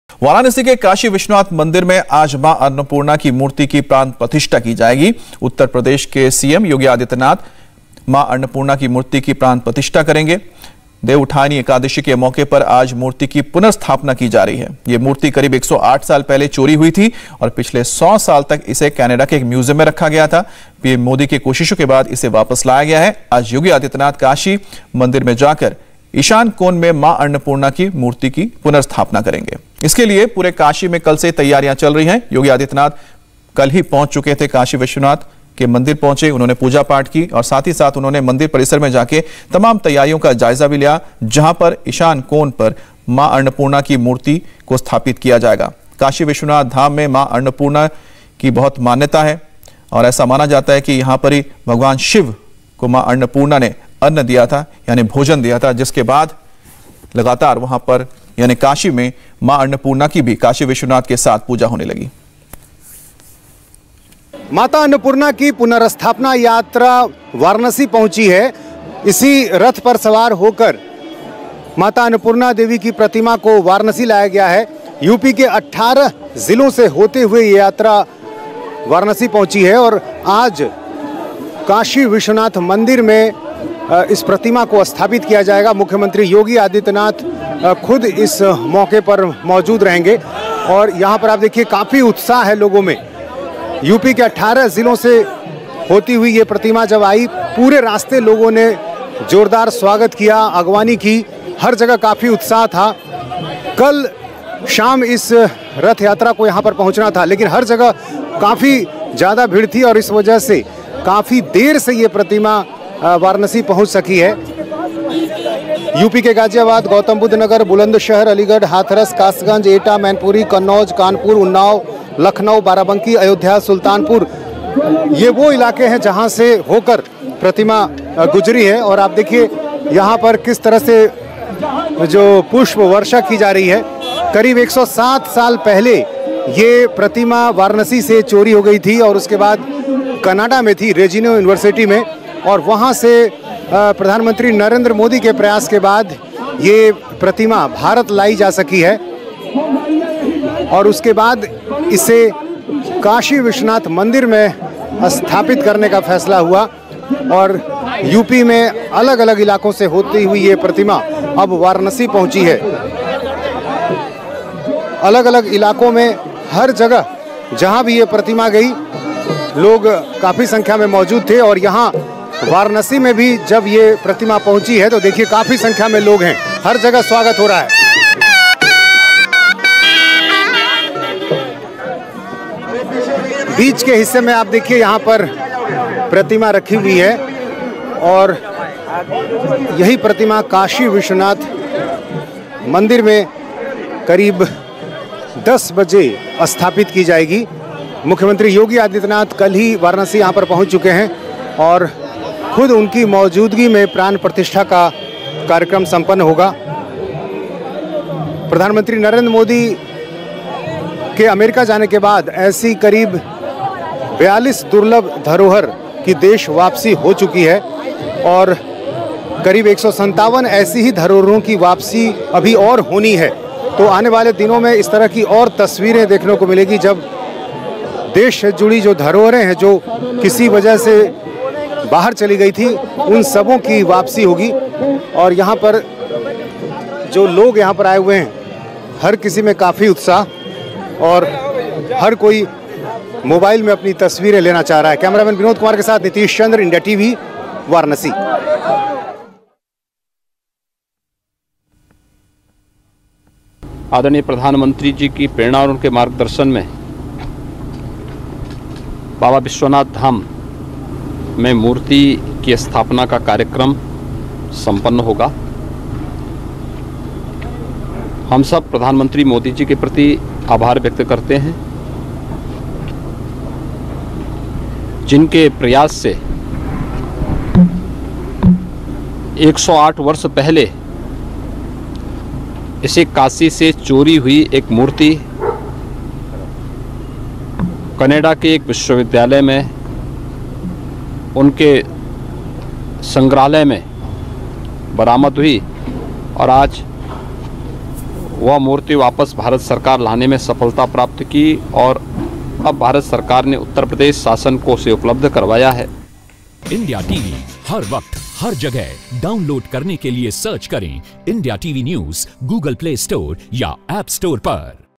Kashi Vishwanath मंदिर में आज मां अन्नपूर्णा के प्राचीन प्रतिमा में स्थापना, देखिए कैसी है तैयारी
आज करीब 100 साल पहले चोरी हुई काशी की प्राचीन मां अन्नपूर्णा मूर्ती की फिर से स्थापना CM Yogi के हाथ से होगी। इस मूर्ती की शोभायात्रा 18 ज़िलों से होकर गुजरेगी।